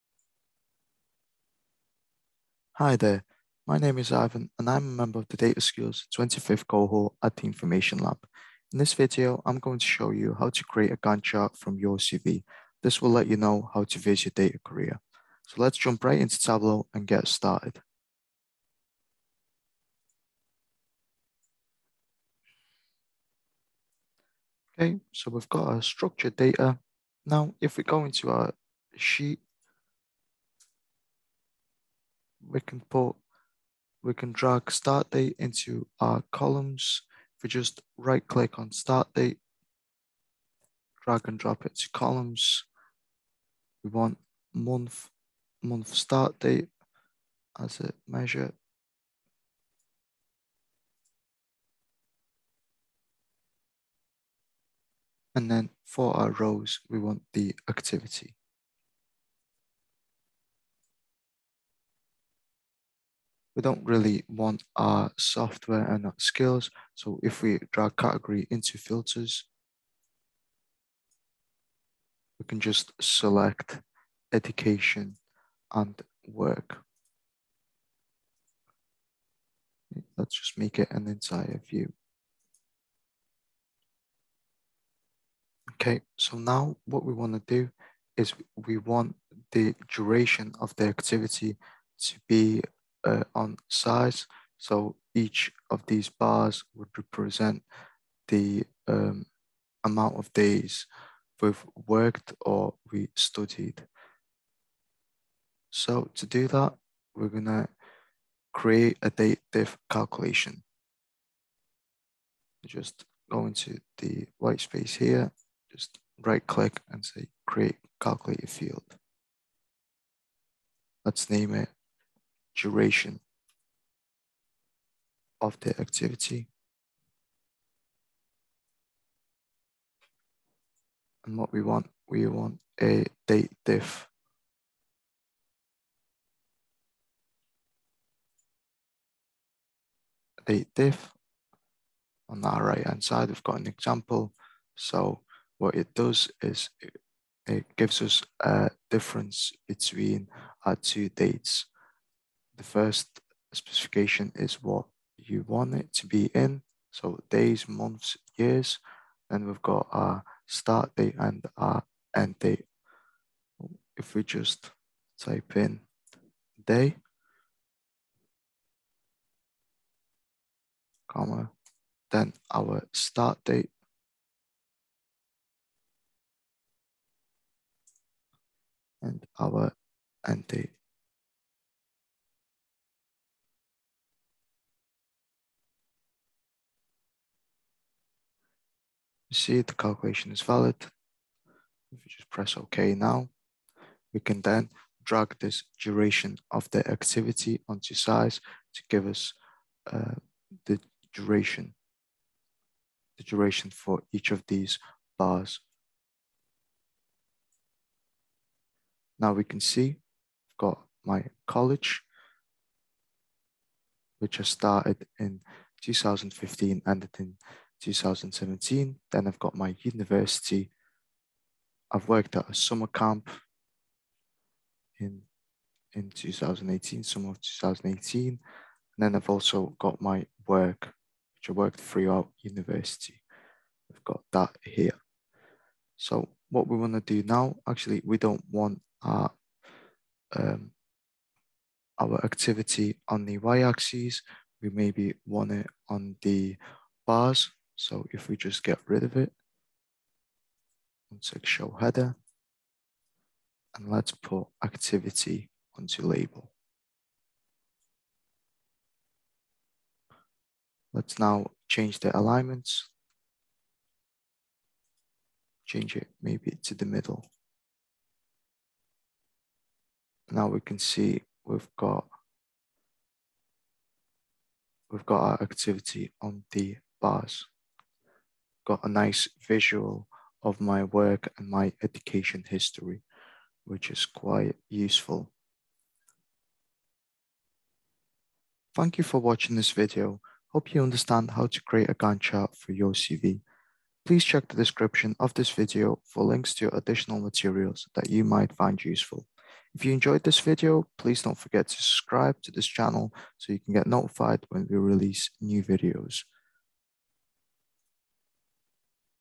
How to in Tableau in 5 mins: Create a Gantt Chart
Learn how to structure your data and create different chart types in Tableau. In this video how to create a gantt chart in Tableau with Ivan Duniak.

- This how-to videos were first shown as in our 'How to Visualise your Resume in Tableau' session